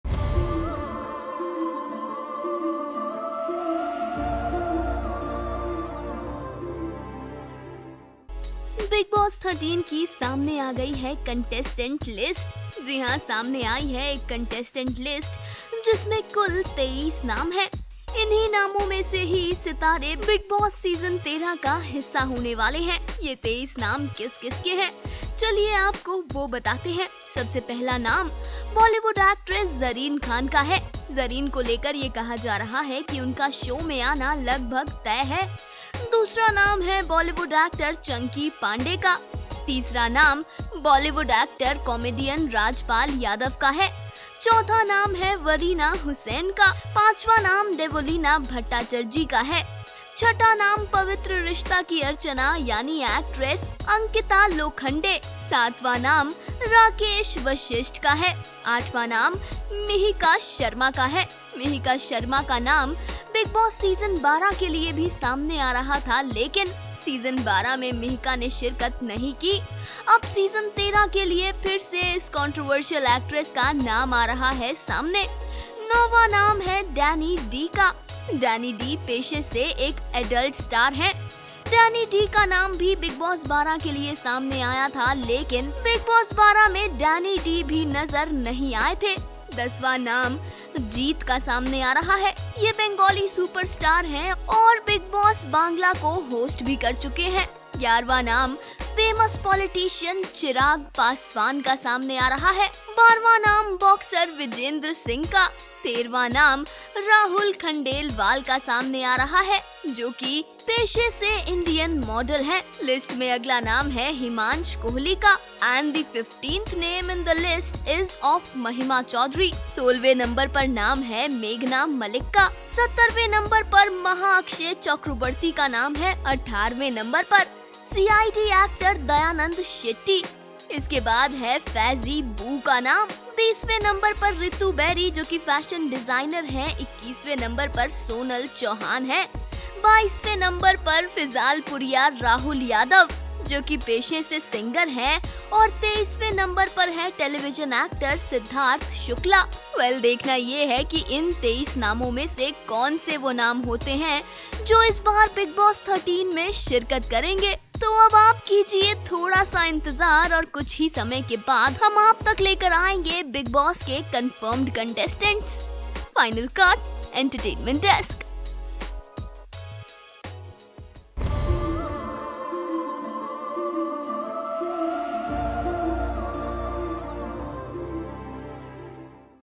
23 Shortlisted Contestant For Biggboss 13| From Actor to Politicians| Biggboss 13 Contestant List
23 Shortlisted Contestant For Biggboss 13| From Actor to Politicians| Biggboss 13 Contestant List